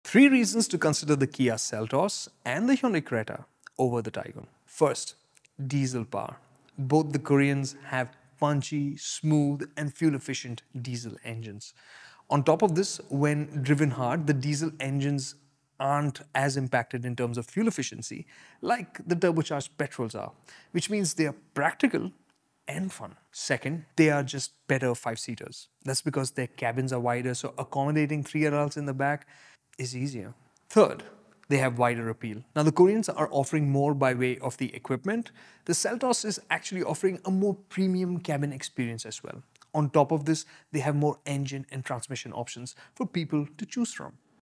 Creta & Seltos vs Taigun | Volkswagen Taigun FAQ #11
Three reasons to consider the Kia Seltos or Hyundai Creta instead of the VW Taigun. First, they have snooth, punchy and frugal diesel engine options. These are also less sensitive to bring driven hard and fast compared to the turbocharged petrols. So, practical and exciting. Second, better five seaters – they have wider cabins which makes them better suited at accomodating three adults in the back. Third, wider appeal– The Korean cousins pack in more equipment, the Seltos has a more premium cabin experience too. And on top of that they have more engine and gearbox options to choose from too.

We’re offering our podcast, ThisConnect, on YouTube, Anchor/Spotify, Apple, Amazon and Google.